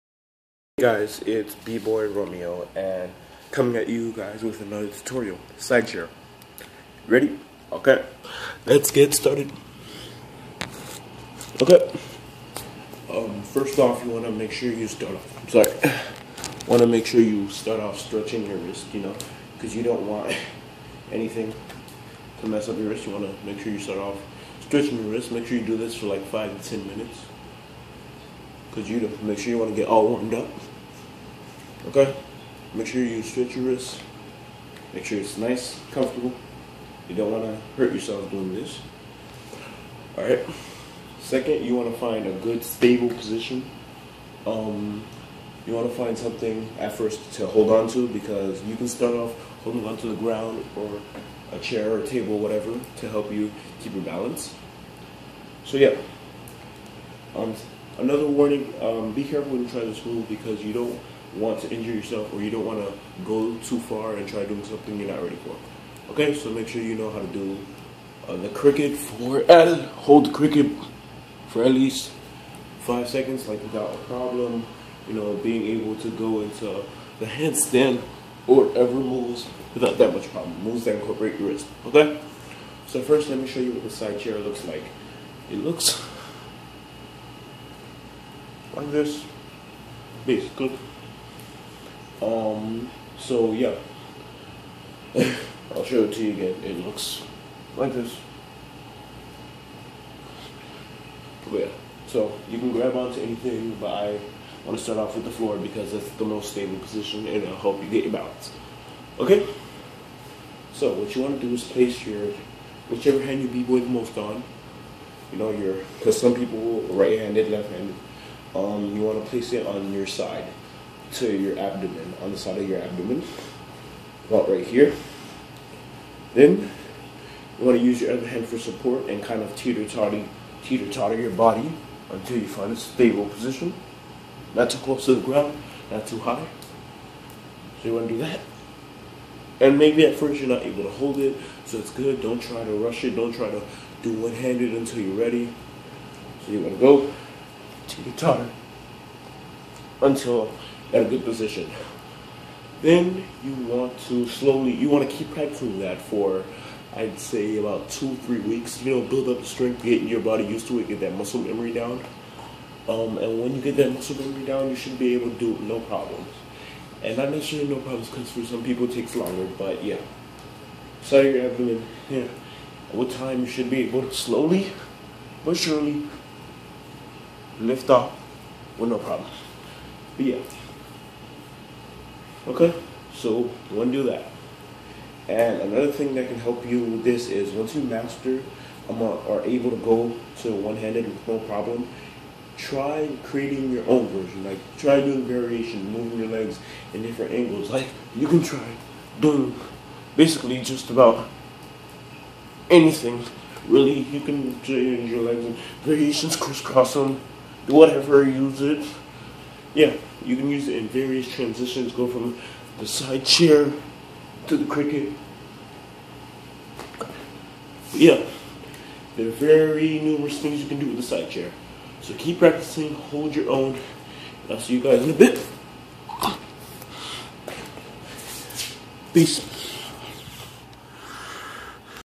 Sidechair Tutorial
A tutorial on the break dancing / bboying move known as the sidechair.